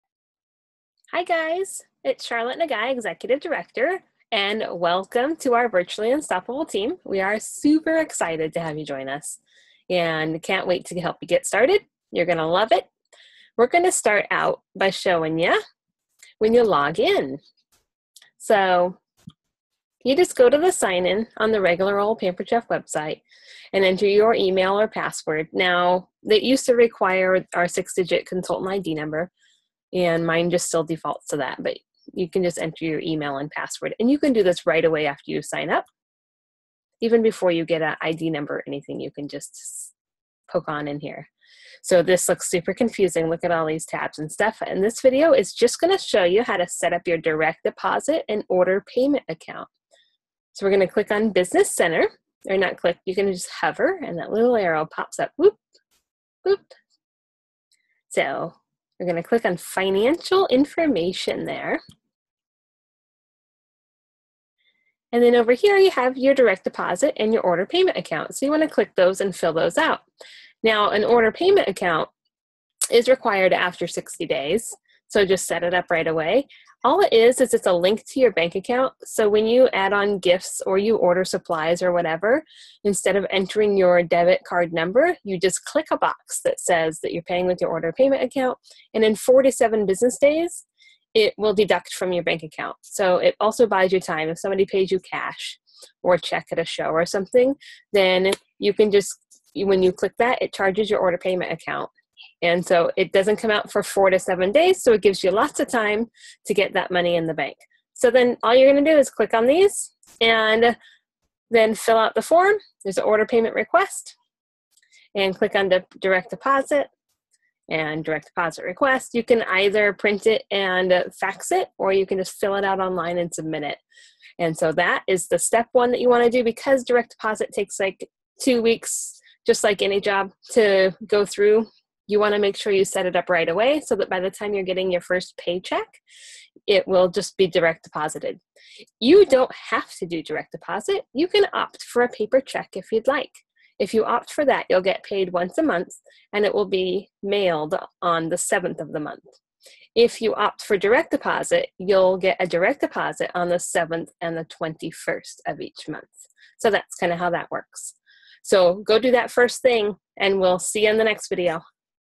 New Consultant Training #1 Welcome and Direct Deposit setup!
CharlottsWeb.com -
Welcome to our Virtually Unstoppable team!  The first thing you want to do is get your Direct Deposit and Order Payment Account setup!